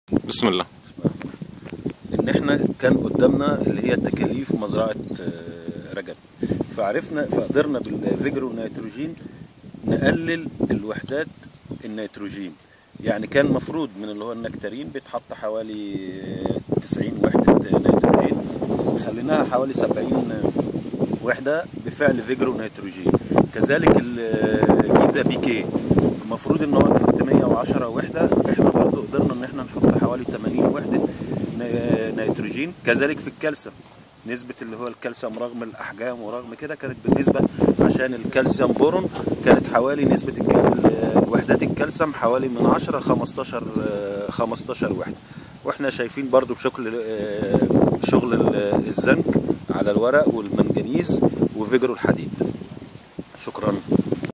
م. طارق سلام: توفير الأسمدة في تسميد النكتارين ببرنامج فيجرو.
2013: م. طارق_سلام، جيزة تك: لتقليل التكاليف بمحصول النكتارين، تم تقليل وحدات النيتروجين بإستخدام فيجرو نيتروجين ووحدات البوتاسيوم بإستخدام جيزة PK ووحدات الكالسيوم بإستخدام جيزة CaB و يظهر على الأوراق التأثير الجيد لإستخدام فيجرو زنك وفيجرو منجنيز وفيجرو حديد. التصوير: بمزارع _رجب، شركة النوبارية للتنمية الزراعية، مصر في 2013.